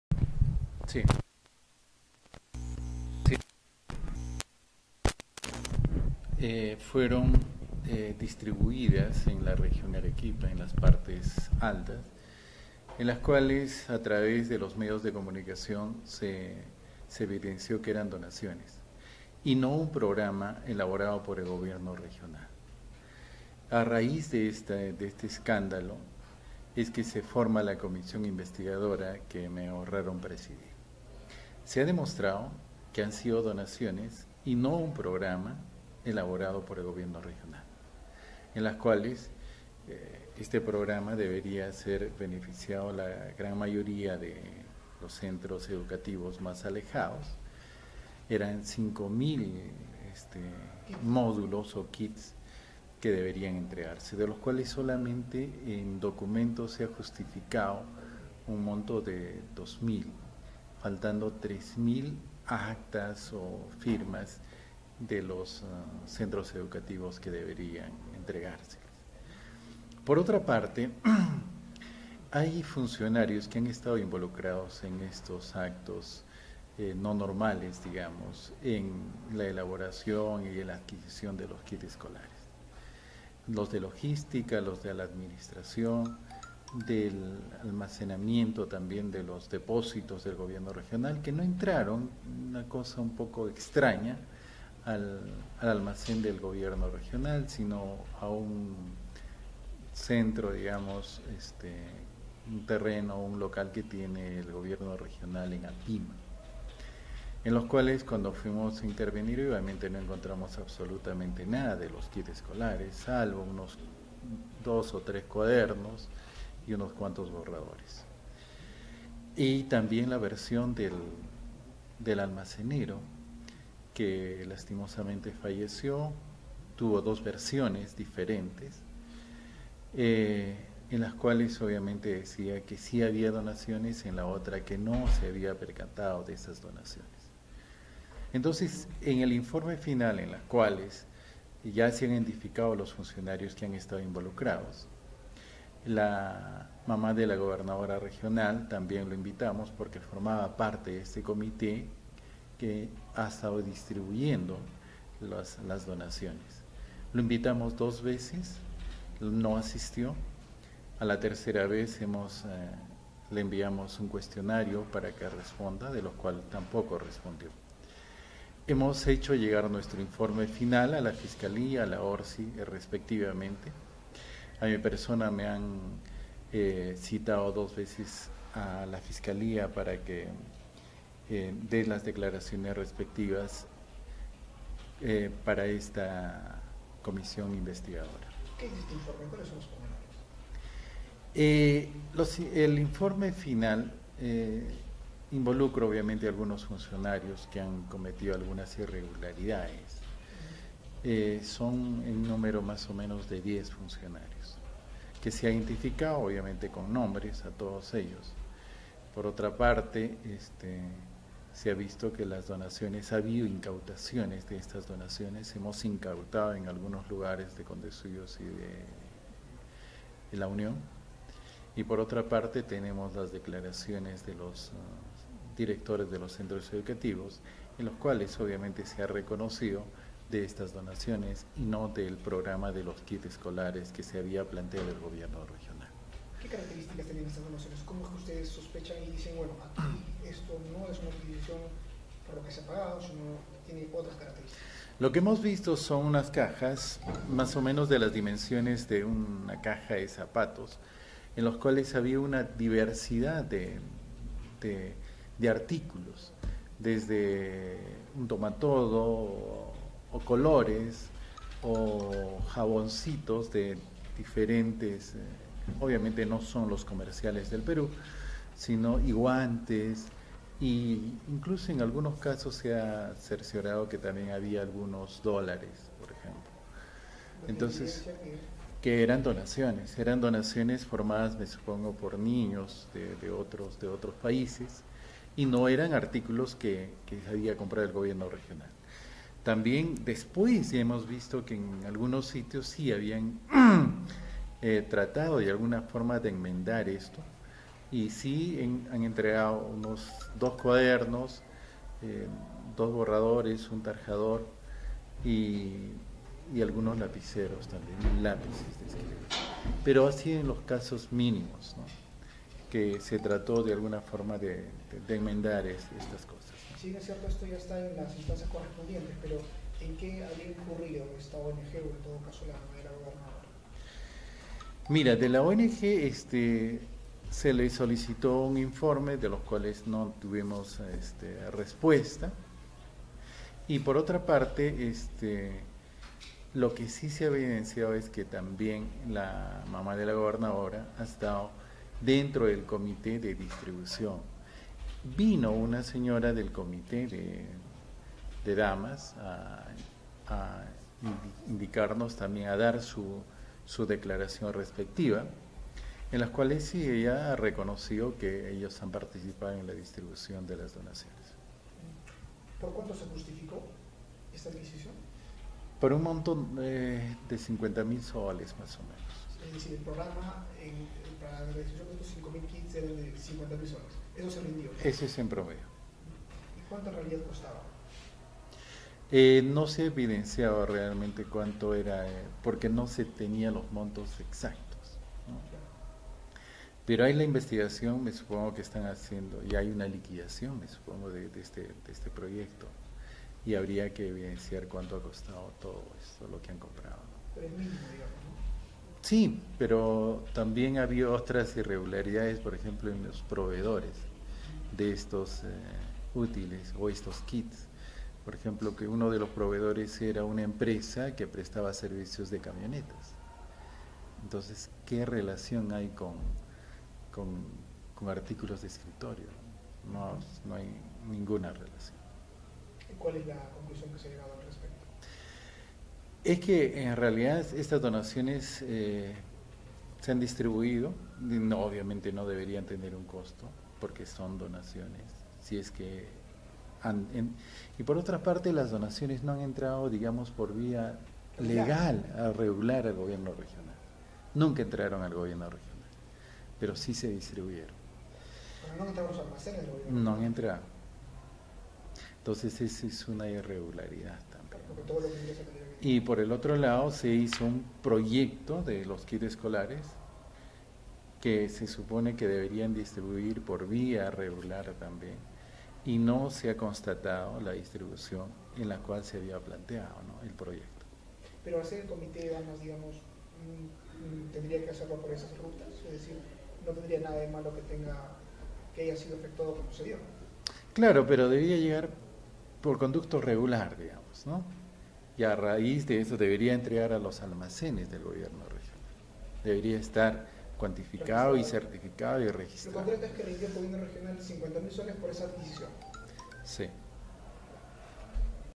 Contraloría y Fiscalía se hacen de la vista gorda con caso de kits escolares
Han pasado tres años y no ha ocurrido nada. Jacinto Rosas, consejero regional de Condesuyos y presidente de la comisión investigadora del caso kits escolares, aseguró que la Contraloría y la Fiscalía no han hecho nada por sancionar a los responsables.